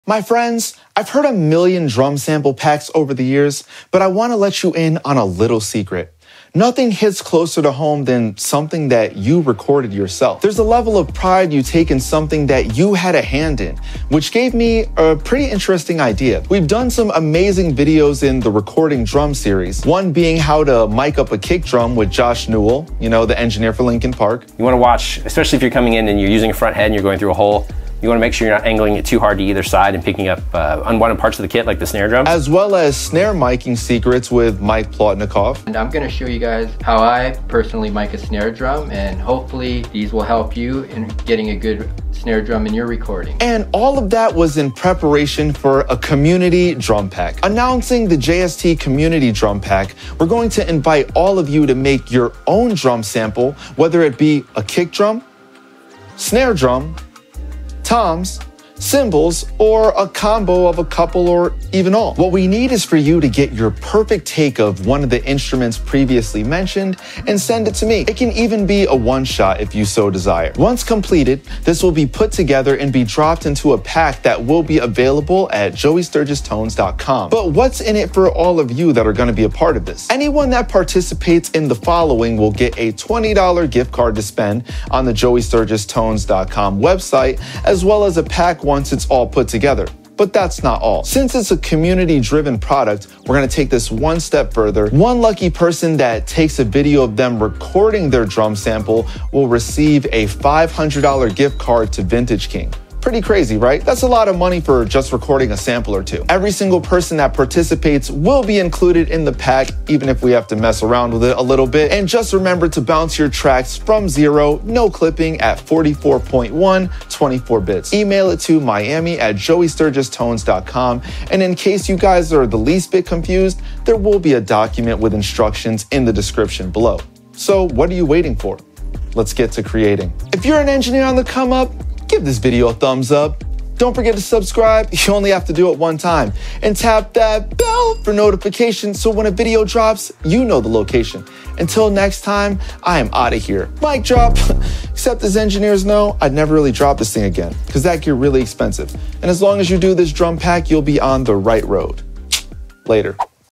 How To Make A Drum Sample Library
Miami asks you to make your own drum samples and submit them to be included in the JST Community Drum Pack! What ideas can you come up with?

Update: All prizes will be sent out april 15. We wanted to allow more people to submit their entries.